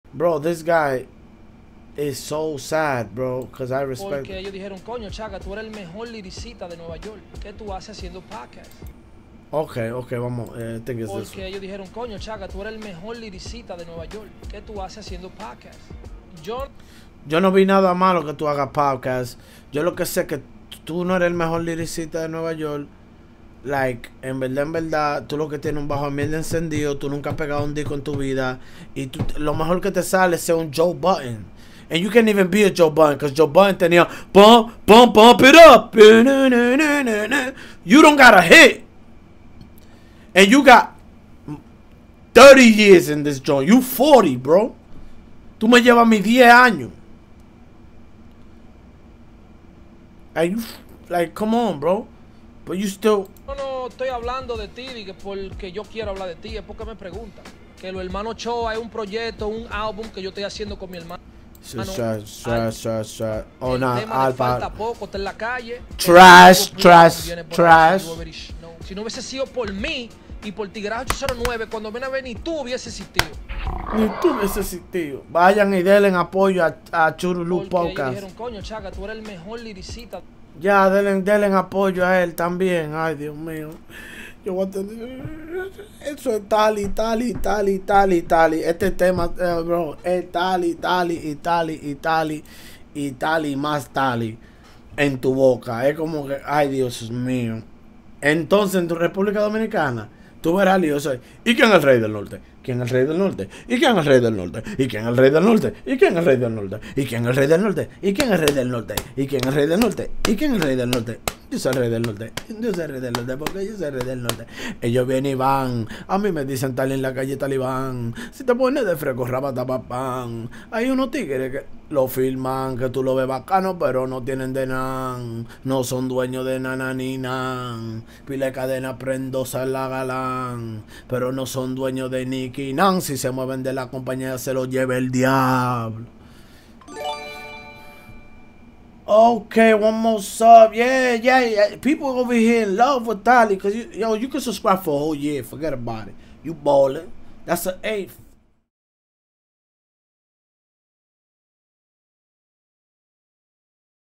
TALI GOYA REACIONA A CHAKA EN SU PODCAST
Tiger Gang, LLC | Channel Managment by SecretStar Distribution.